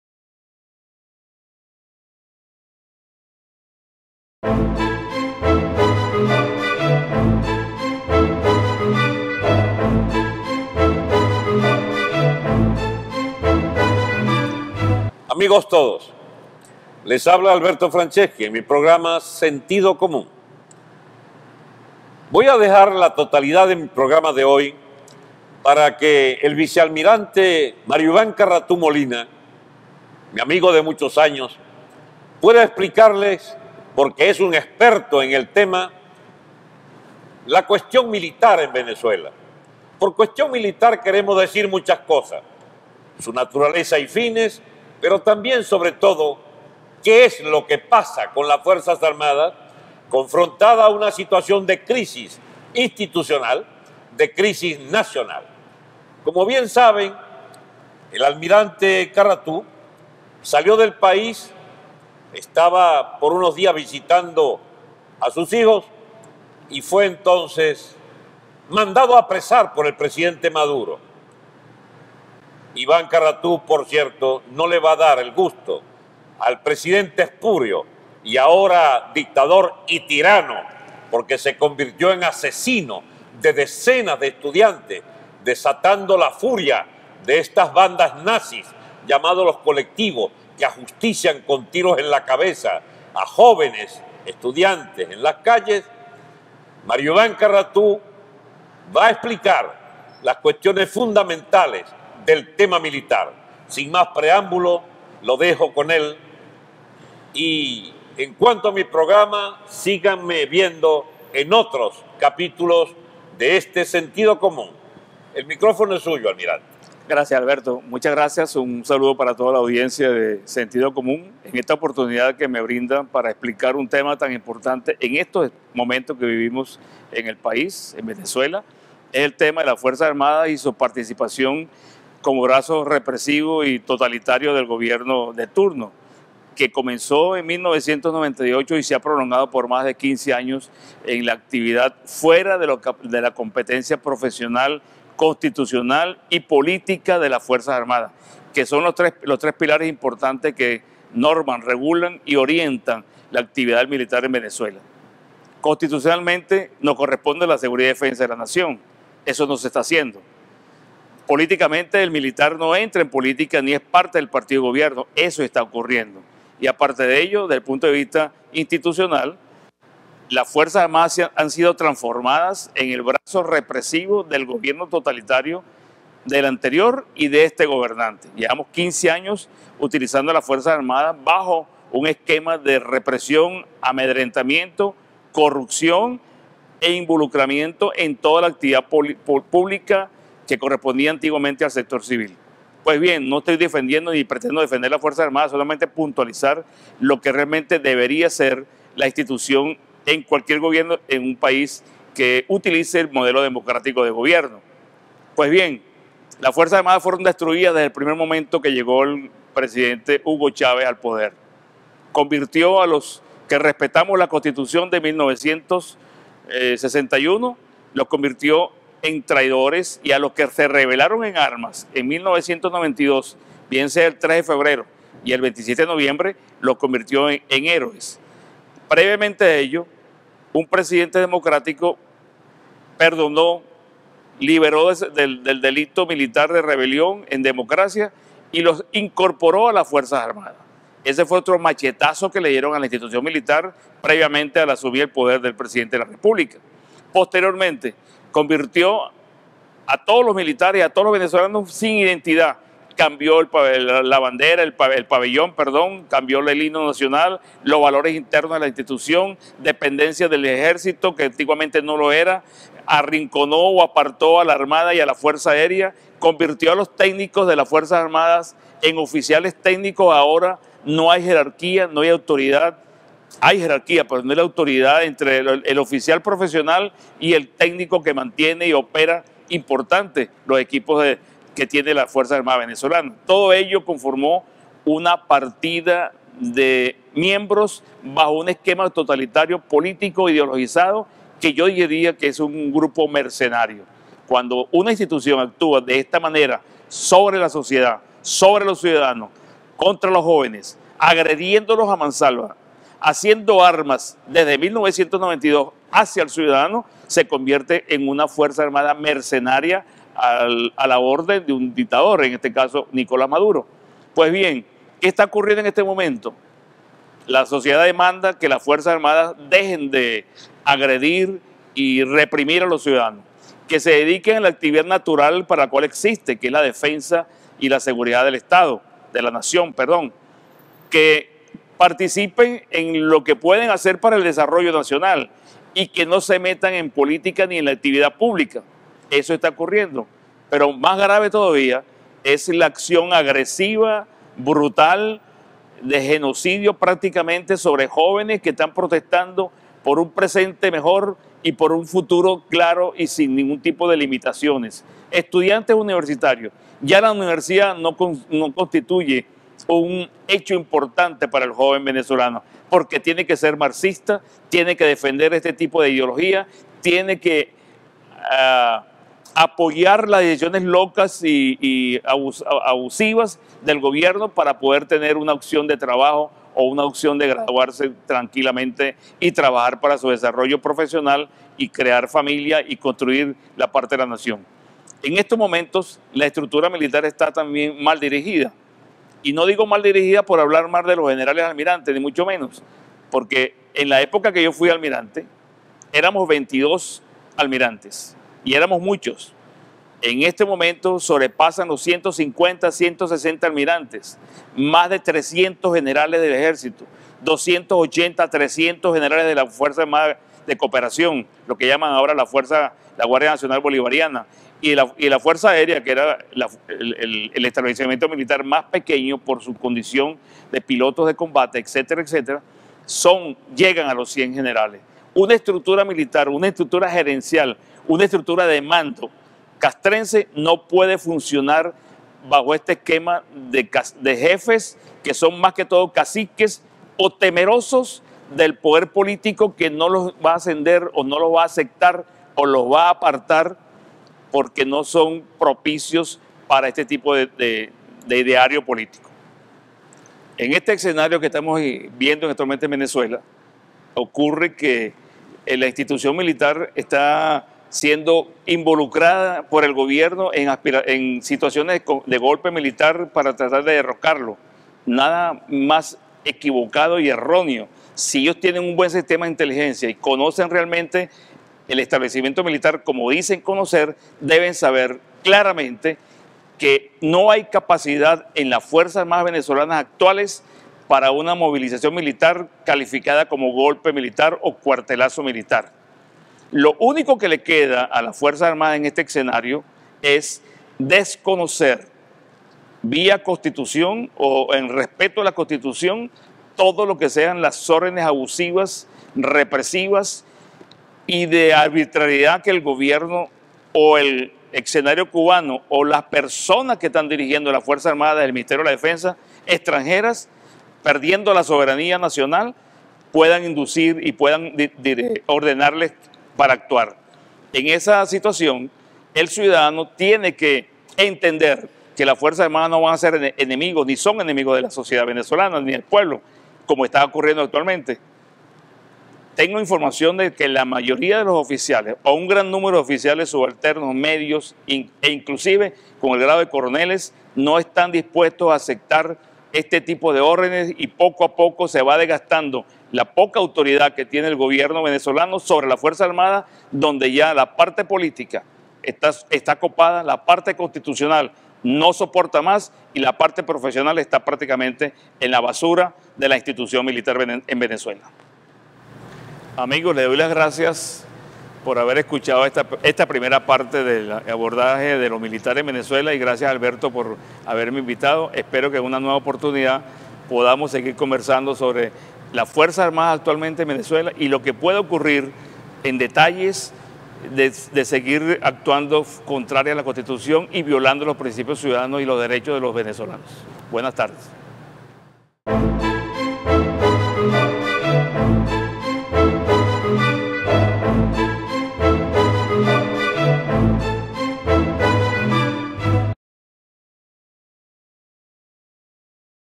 Sentido Común #32 - Mario Ivan Carratú Molina - La cuestión militar en Venezuela
El Vice Almirante Mario Ivan Carratú Molina quién fue mandado a apresar por el gobierno de Nicolás Maduro expone la situación del tema militar en Venezuela.